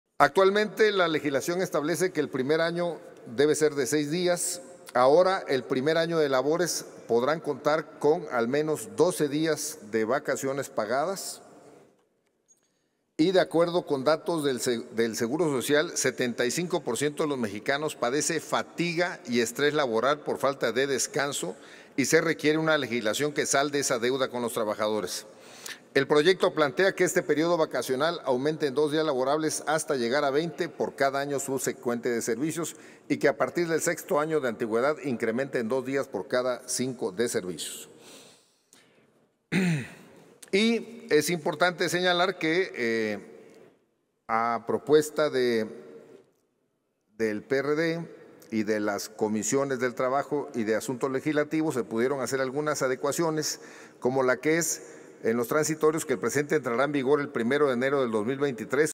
Juan Manuel Fócil (PRD) durante la discusión de la reforma sobre vacaciones dignas
Escucha parte de la intervención del senador Juan Manuel Fócil, del Grupo Parlamentario del PRD, durante la discusión de la reforma para duplicar de 6 a 12 días el periodo vacacional de las y los trabajadores.